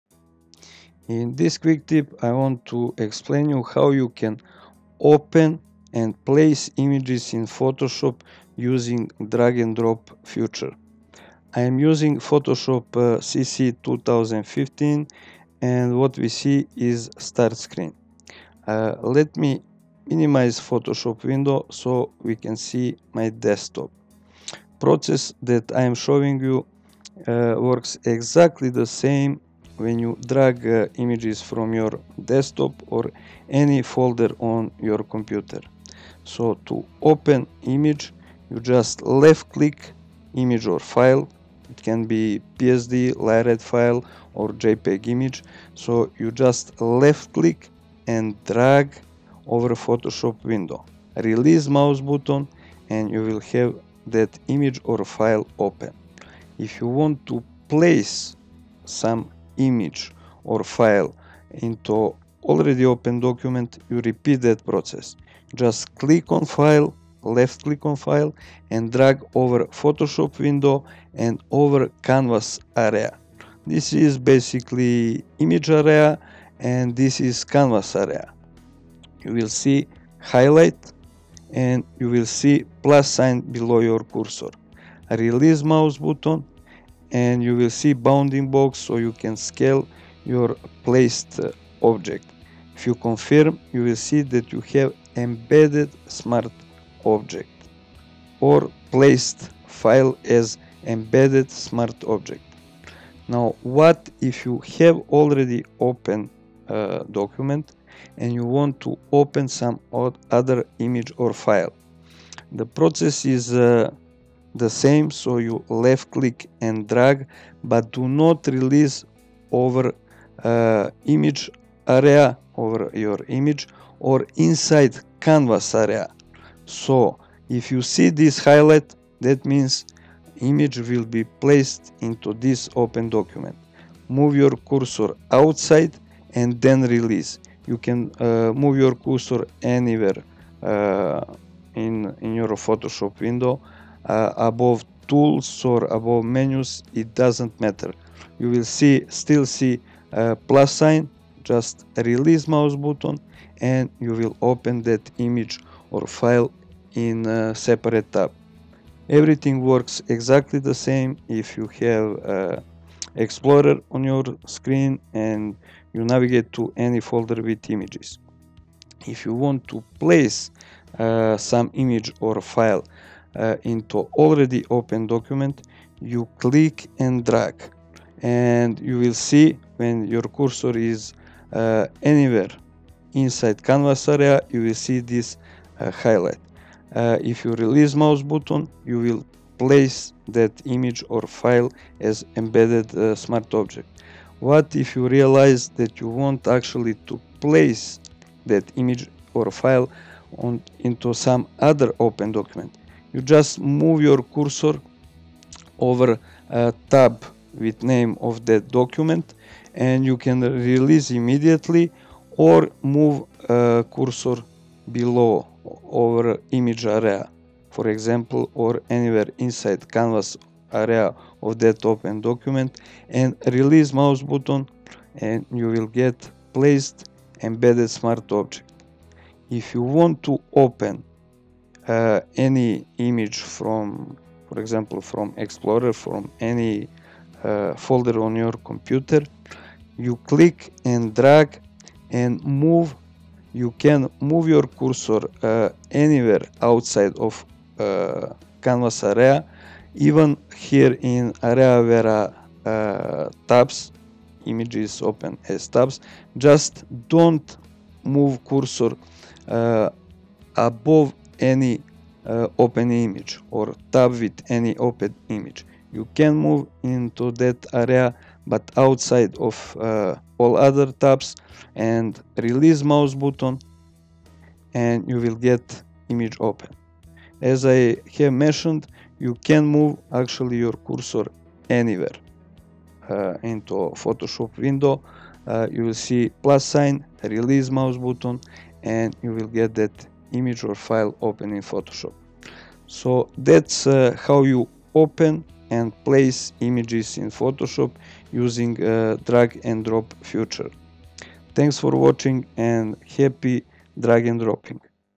Drag and Drop to Open or Place File in Photoshop
Quick tip with tricky part how to open and place image in Photoshop using popular drag and drop feature.